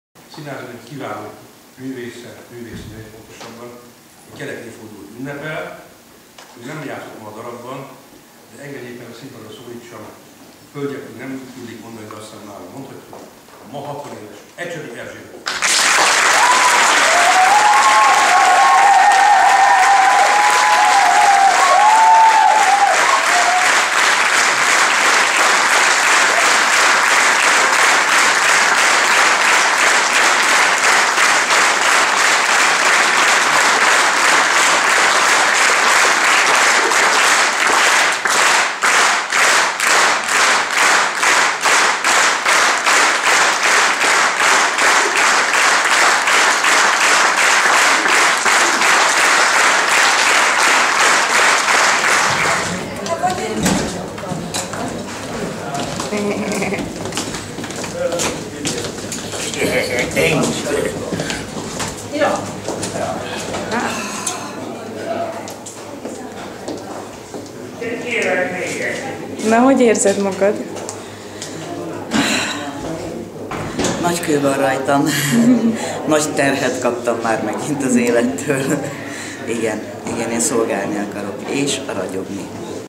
Ecsedi Erzsébet születésnapi köszöntése
Ecsedi Erzsébet 2014. április 24-én töltötte be 60. életévét. Besenczi Árpád és Sztarenki Pál a színpadon köszöntötte a művésznőt, a mi Ecsinket. Az igazgató felolvasta Szigethy Gábor születésnapi köszöntő levelét. A színháztörténész Ecsedi Erzsébetre a Színésznők című előadásban nyújtott alakításakor figyelt fel.

Szigethy Gábor születésnapi köszöntése:

Hölgyeim és Uraim!

Öregember vagyok, irigylem a fiatalokat, irigylem a hatvanéves Ecsedi Erzsébetet, azaz Ecsit, aki töretlen hittel, munkakedvvel, örömosztó szenvedéllyel ballagott végig magyar színésznőként, művészként a 20. század utolsó évtizedein. Nem magában hitt, hanem a feladatban. Tudta, ha a sors: „Magyarországra dob ki valakit / Annak százszorta inkább kell akarnia".
 Ecsi életének ez a kulcsmondata: inkább kell akarni. Nem beletörődni, nem beleszomorodni, nem belekeseredni, nem belerokkanni a fojtogató-boldogító mindennapok életsűrű kátyújába. Inkább akarni; akarni játszani, teremteni, alkotni... hasznosan élni...
 A magyar történelem csillagfényes múltjában, a reformkorban sokan -- Berzsenyi Dániel, Széchenyi István, Wesselényi Miklós, Petőfi Sándor -- vallották hittel, elszánt élni akarással: „használni, s nem ragyogni akarok".
 Ecsi sokáig nem ragyogott, csak használt. Tette a dolgát, színésznő volt Magyarországon, munkájával szolgálta a közönséget. Akik látták, tudták: úgy használt, hogy ragyogott. Akik nem látták, nem tudták: használt és ragyogott.
 Öregkorom boldogsága, hogy a fiatal, ma hatvanéves Ecsedi Erzsébetet, azaz Ecsit 2013-ban, a Pécsi Országos Színházi Találkozón így köszönthettem a zalaegerszegi Hevesi Sándor Színház Színésznők című előadásának pécsi bemutatója alkalmából: „Színésznők színésznőket játszanak remekül, a régi színházi történet mai történet, a régi színésznők mai színésznők és Ecsedi Erzsébet színpadi varázsló: reményt, életkedvet, hitet fakaszt lelkünkben, szívünkben."
 Ecsi, drága! Ragyogj és használj, légy, amíg élsz, boldog szolgálólánya a színpadnak, nemzetednek, hazádnak.
 Csókolom a szívedet, Isten éltessen sokáig!

2014. április 24.

         Szigethy Gábor